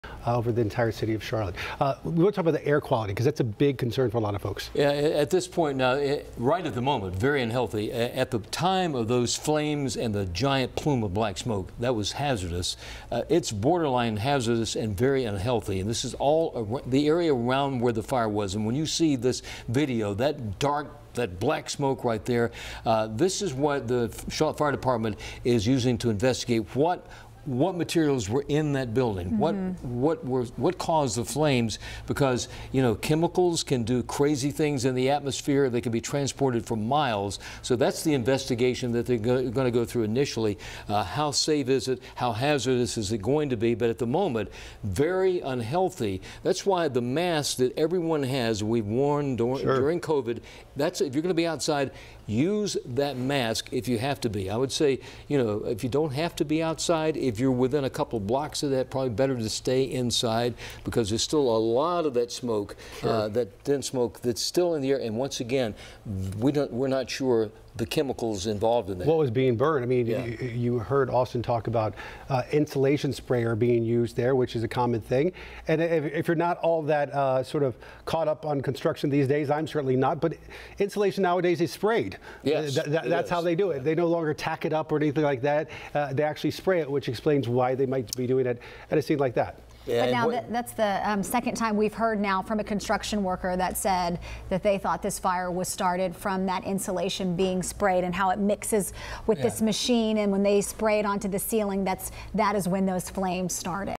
Air quality 'very unhealthy' after massive Charlotte fire
Residents are urged to stay indoors and away from the south Charlotte fire because of concerns of materials that could be burning into the air.